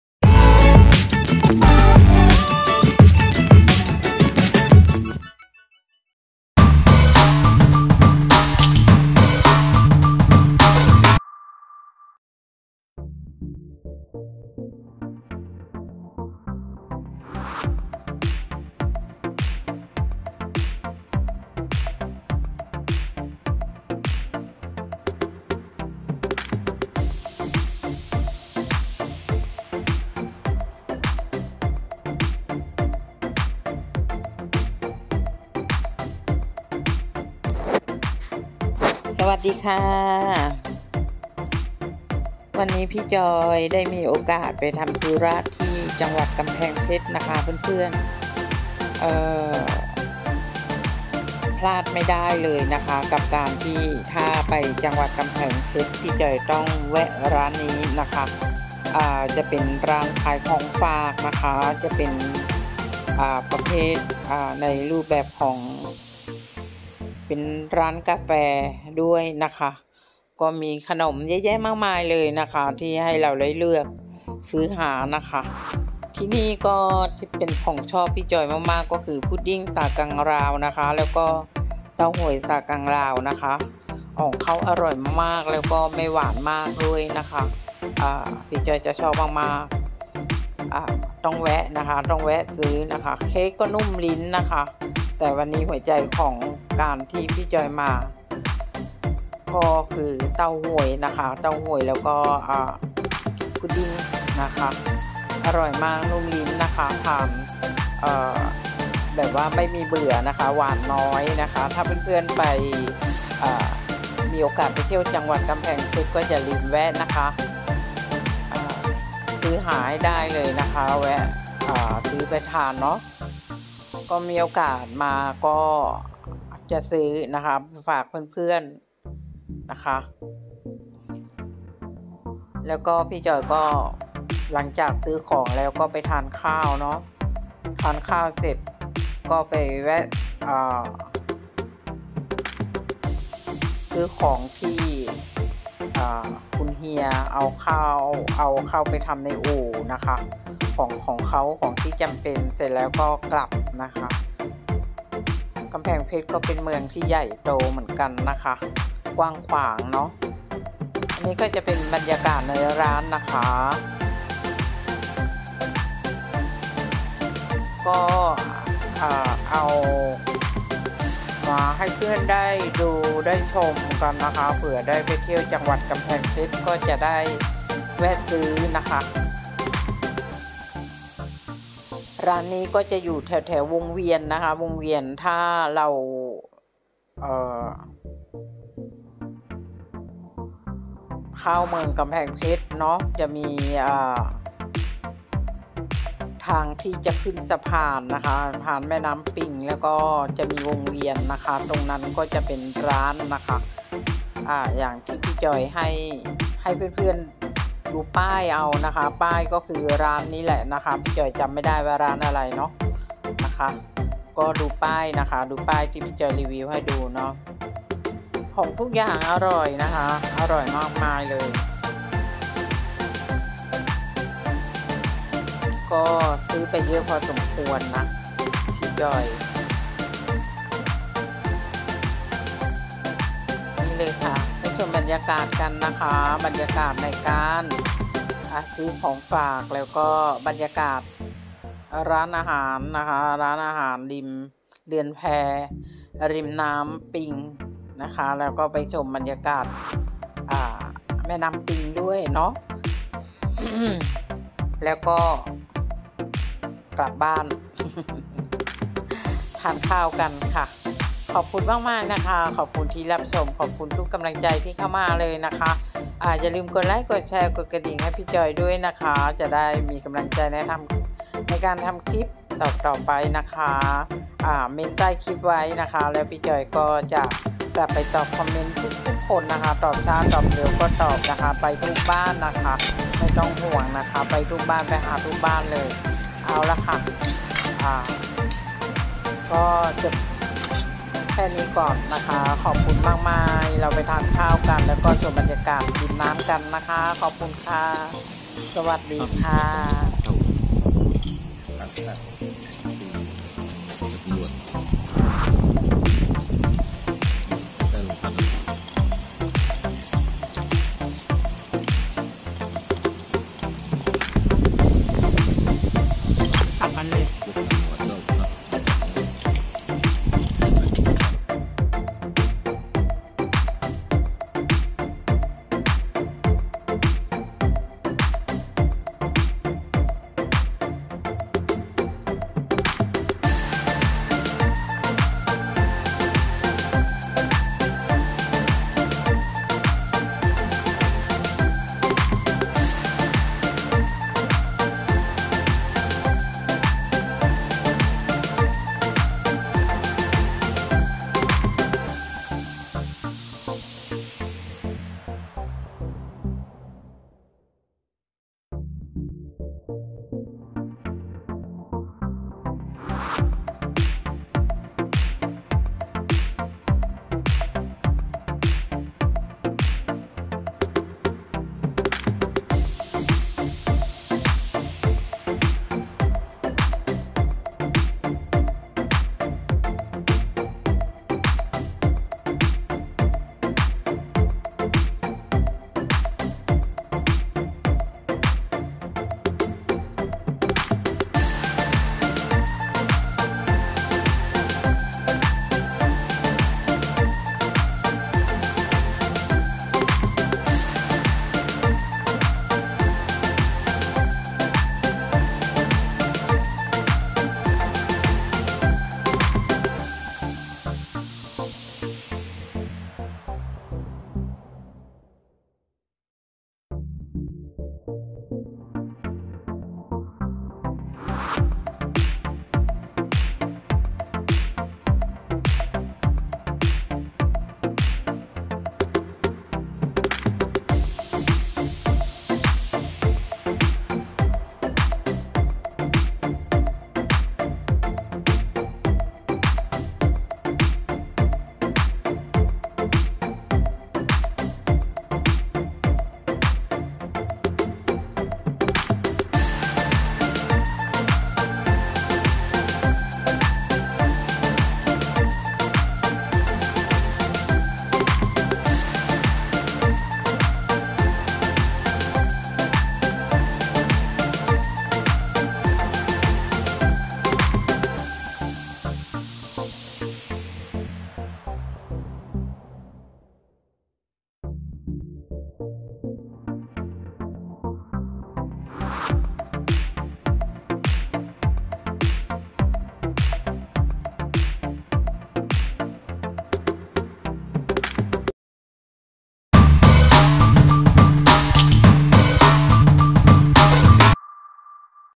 จ.กำแพงเพชร#22/3/64
สวัสดีค่ะพี่จอยไปธุระ จ.กำแพงเพชรค่ะ มาชมบรรยากาศซื้อของฝากทานข้าวริมปิงกันค่ะ ขอบคุณที่รับชมค่ะ❤️